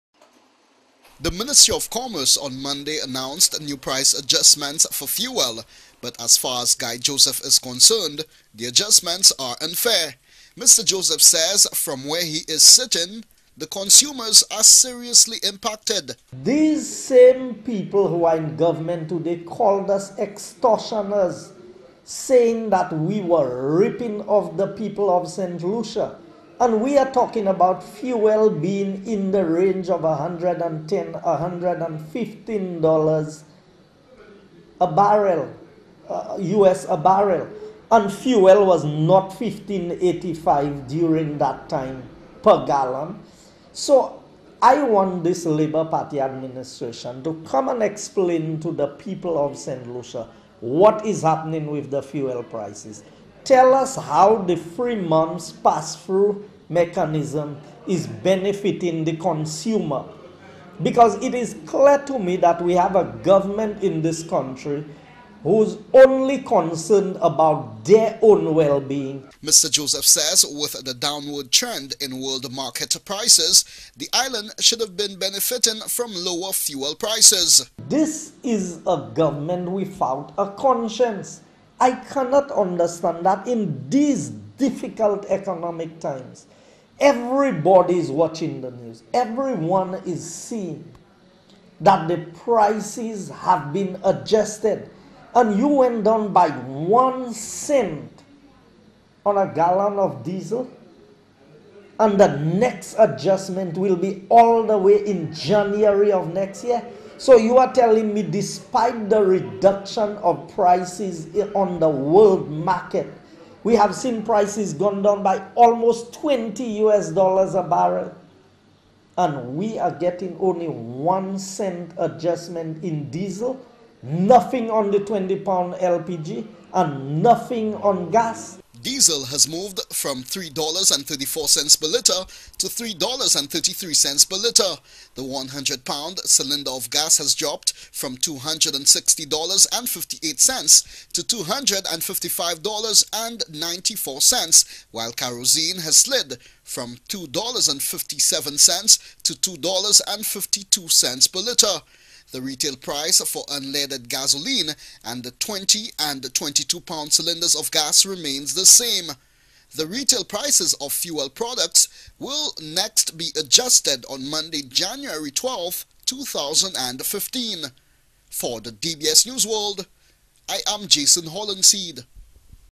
UNITED WORKERS PARTY HON GUY JOSEPH RIP SLP ON GAS PRICES
United Workers Party Castries SouthEast Hon Guy Joseph Calls on the Labour Party Government to let gas prices reflect the price of oil on the world market.